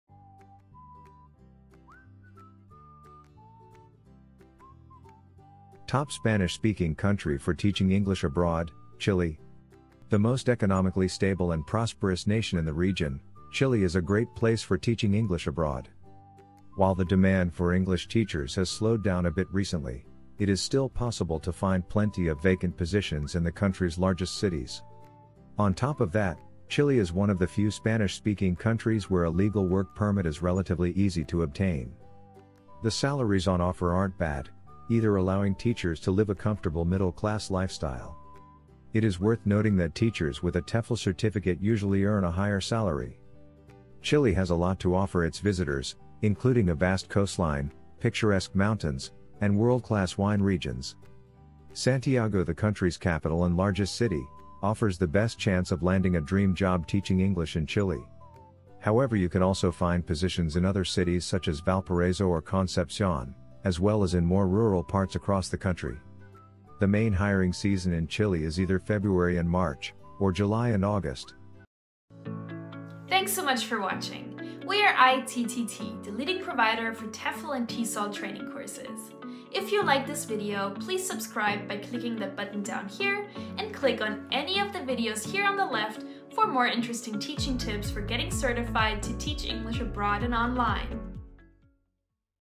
Teaching English Abroad: Chile
Teaching English Abroad. Do you want to teach English abroad? Here are the top 7 Spanish countries to work as an English teacher.